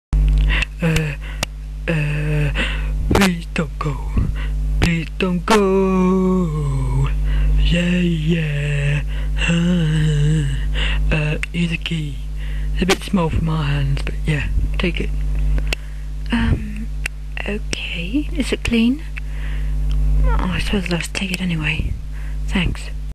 HG Voice Over!
XD Yes a lil project am working on! Still need to make really funny ones but hey I isnt funny on the spot! So here is a scene with Debilitas and Fiona! Please enjoy! And yes my impressions suck but hey! Least I try to make you's guys laugh! =P